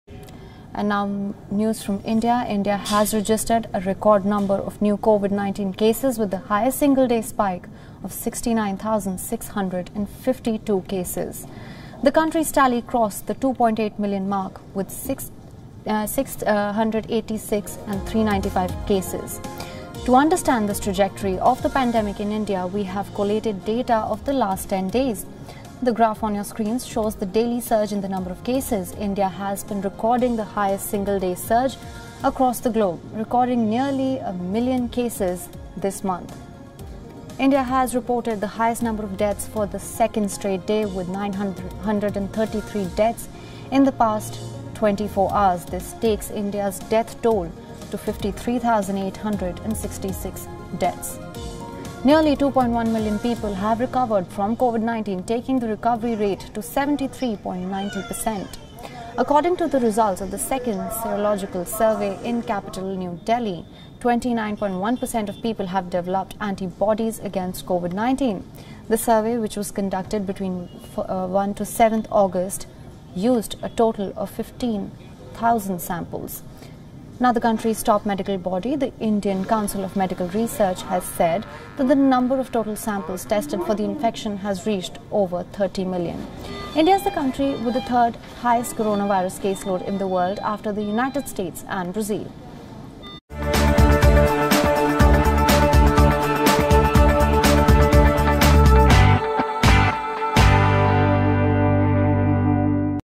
India COVID 19 tracker Nearly 70,000 new cases and 977 deaths in last 24 hours
India has registered a record number of new COVID-19 cases with the highest single-day spike of 69,652 cases. The country's tally crossed the 2.8 million mark.